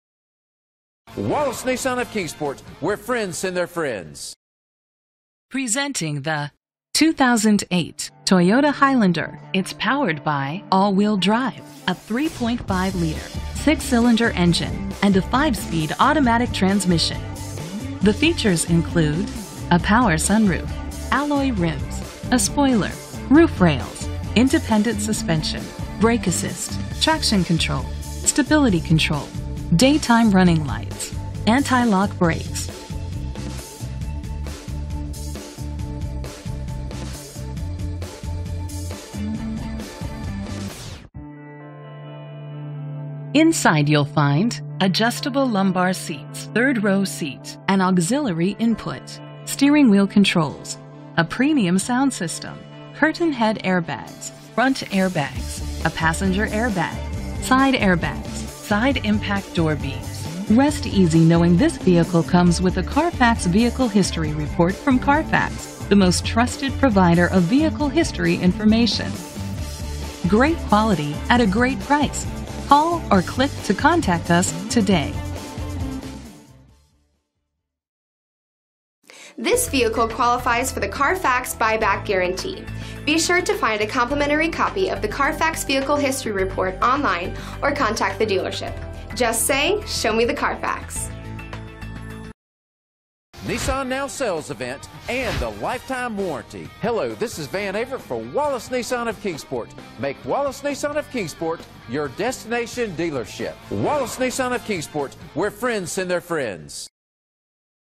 2008 Toyota Highlander 5253P - Kingsport TN
Phone:
Year: 2008
Make: Toyota
Model: Highlander
Trim: Sport
Engine: 3.5 liter 6 Cylindercylinder AWD
Transmission: 5-Speed Automatic
Color: Dk. Red
Mileage: 123103
Address:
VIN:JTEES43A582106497